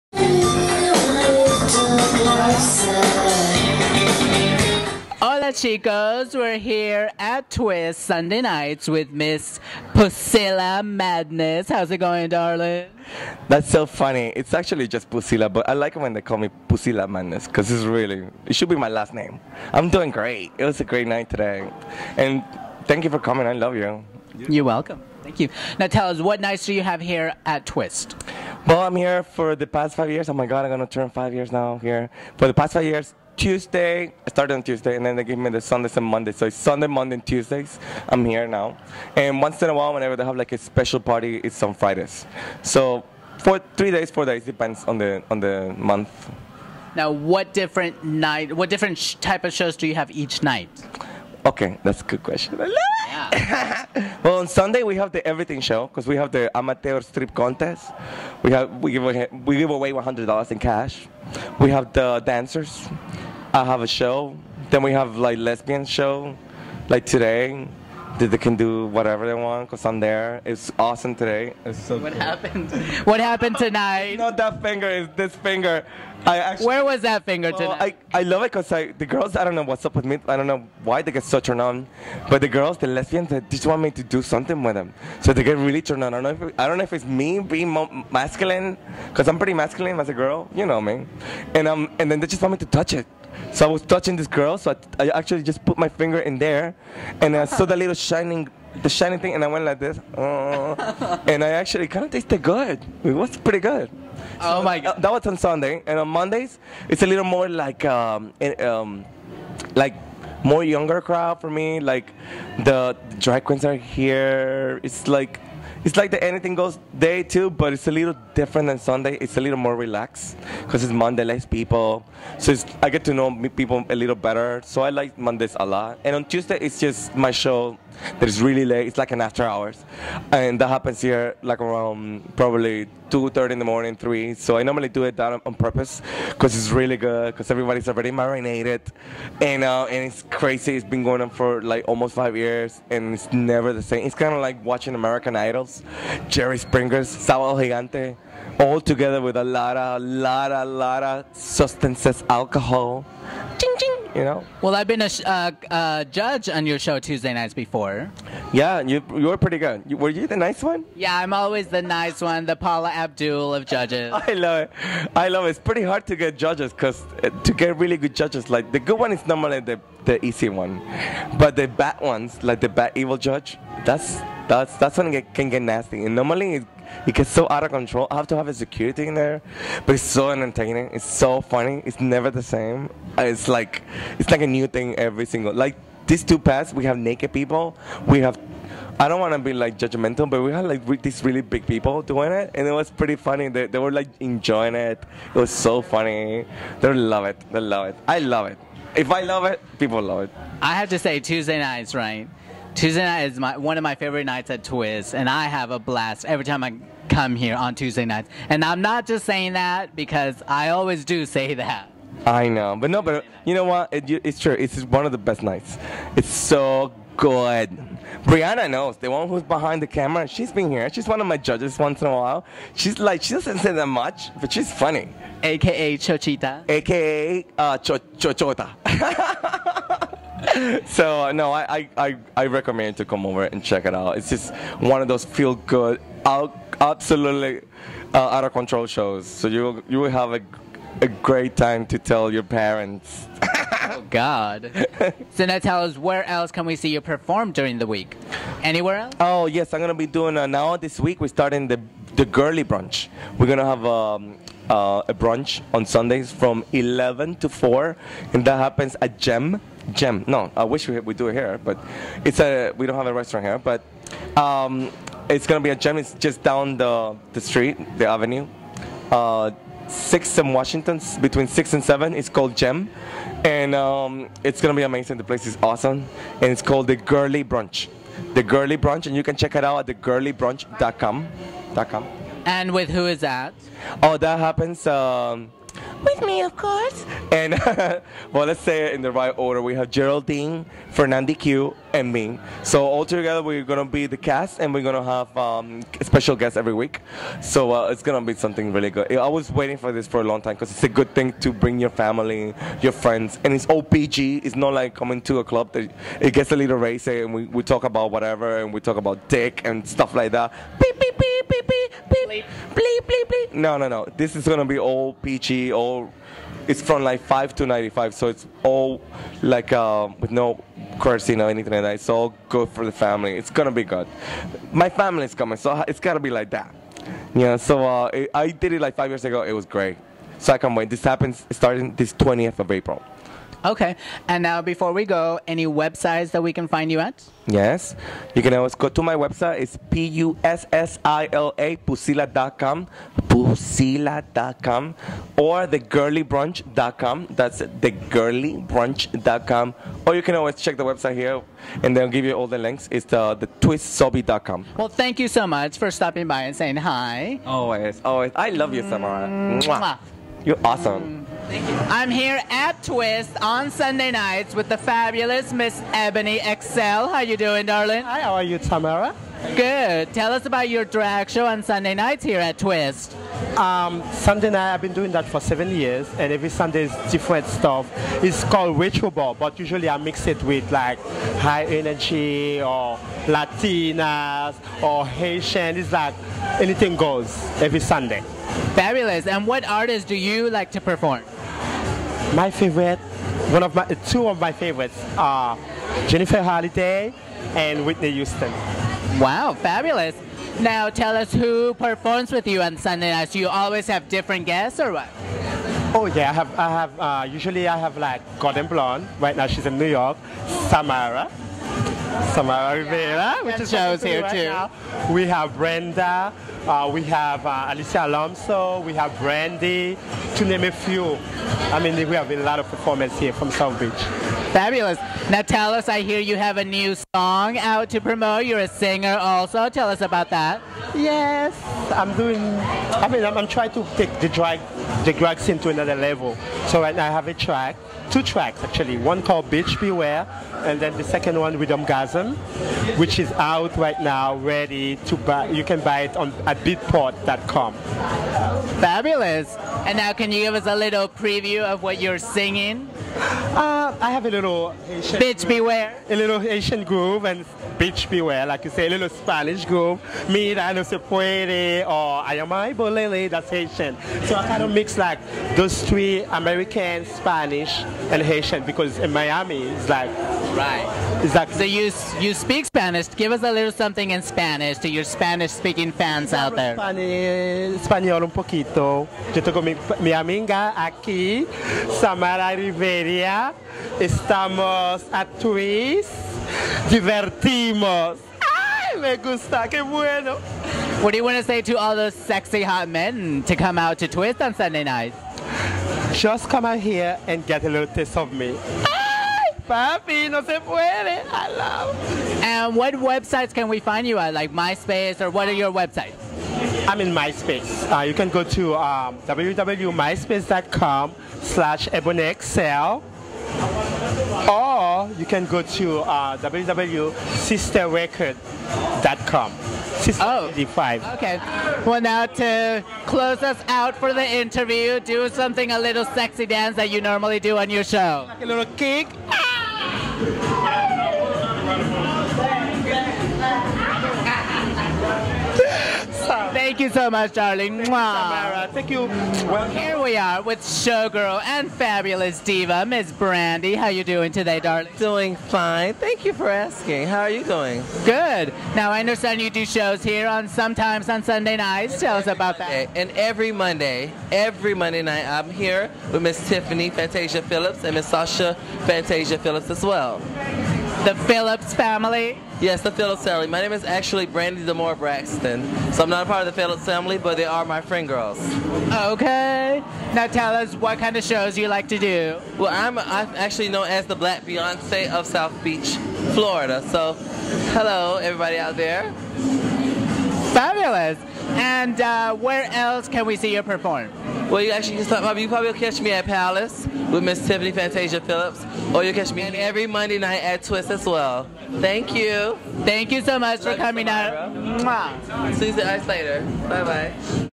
La Riviera @ Twist w/ Pussila/Ebonee Excell drag queens!
transgender journalist samara riviera at south beach's gay club twist interviews pussila and ebonee excell drag queens about their sunday night shows
april 08'
la reportera transgenero samara riviera visita el gay antro de south beach y entrevista a las travestis pussila y ebonee excell abril 08